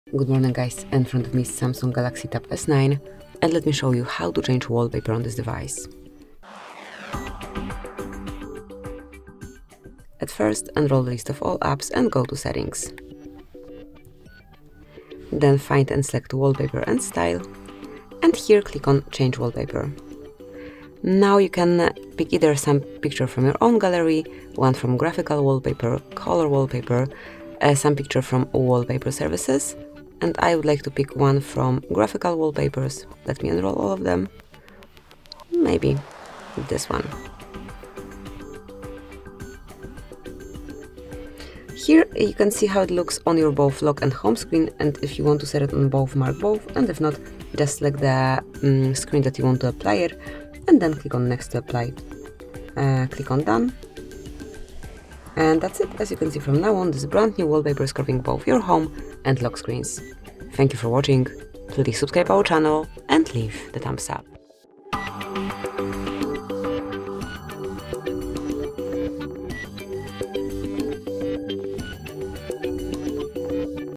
How to Change Wallpaper on Samsung Galaxy Tab S9?
Learn how to easily change the wallpaper on your Samsung Galaxy Tab S9 with this step-by-step tutorial video. Customize your device's look and feel by exploring the simple process of setting a new wallpaper that suits your style and preferences.

How to Replace Wallpaper on Samsung Galaxy Tab S9?
How to Adjust Home Screen Background on Samsung Galaxy Tab S9?
How to Set a New Wallpaper on Samsung Galaxy Tab S9?
How to Update Home Screen Image on Samsung Galaxy Tab S9?